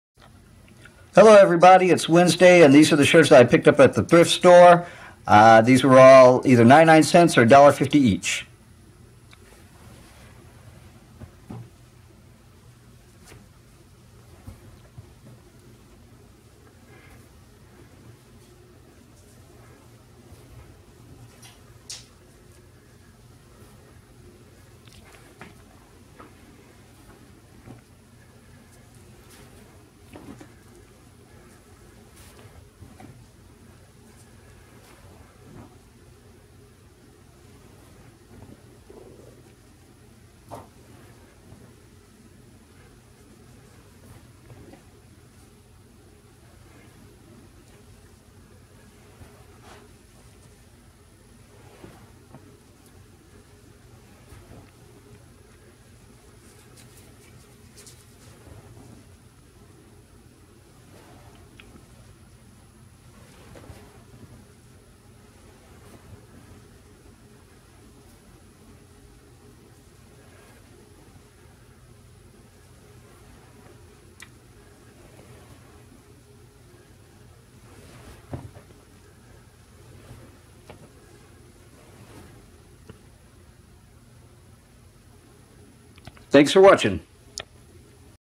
Thrift Store Finds #2 Concert Band Tour Shirts
More shirts found at the thrift store. I give these to a friend who likes to collect cool stuff!
These cost 99 cents to $1.50.
RULE ONE: NEVER pay more than $1.99 for a shirt.